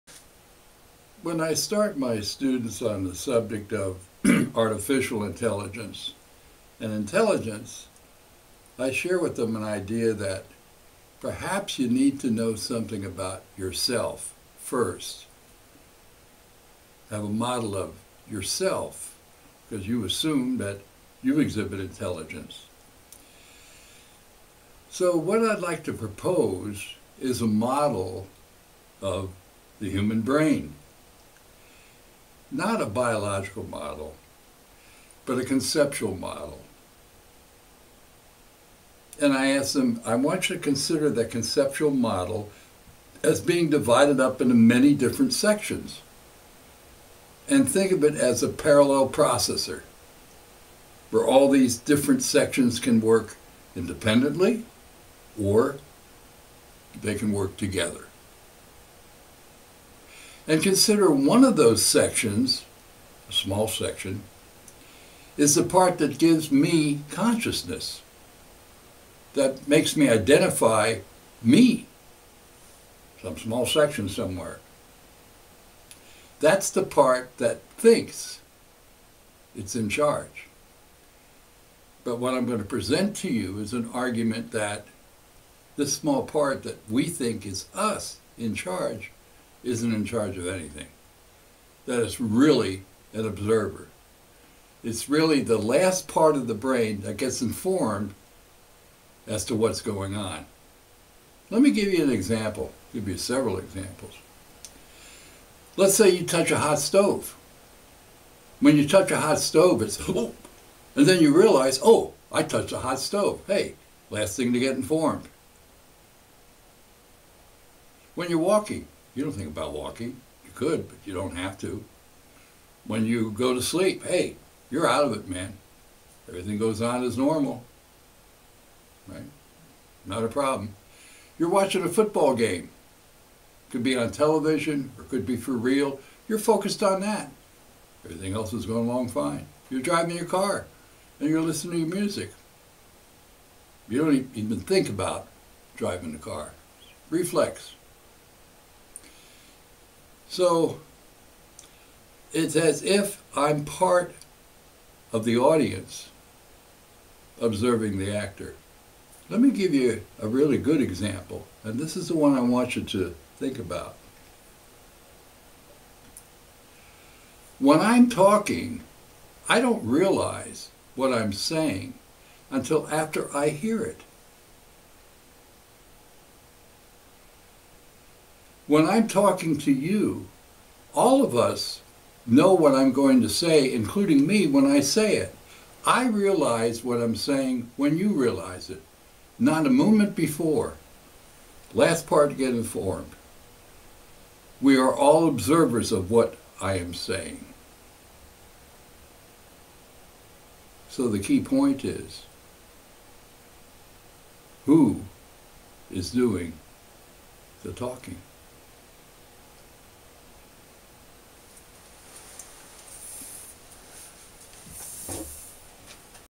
AI Challenges - Who is doing the talking?
Describing a model of the human brain as a parallel processor consisting of many components, one of which is the concept of self, the last part to be informed.